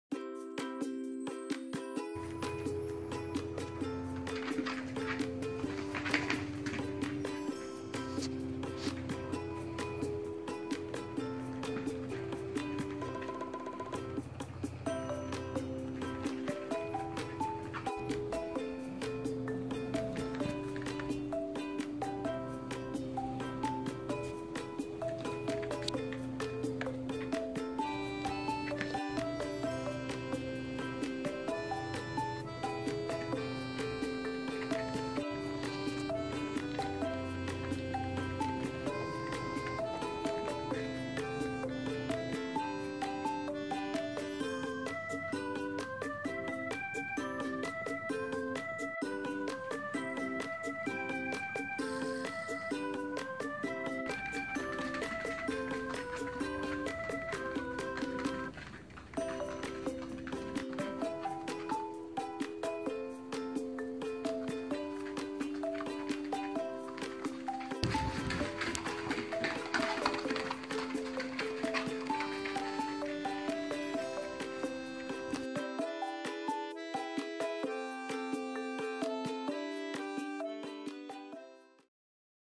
いちばん年上だけど、甘えちゃいまーす！【ひみつきちなう！②】2018年1月30日
甘え上手なチワックス”チョコちゃん”なのでした。

コーギー　ルイくん
ミックス犬　ザックくん
チワックス　チョコちゃん
柴犬　こまめくん
トイプードル　ハムレットくん